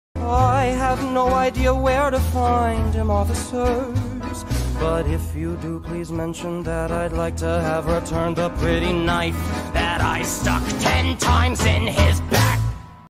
TEN TIMES IN HIS BACK meme
tw blood, death
original by HolyChip

i think it's the first time i've shown mariella's redesign
also impulsive animating
anyway she kills sha over the multiverse because he absolutely deserves it

characters-
mariella- white elf lady
sha- dead angel guy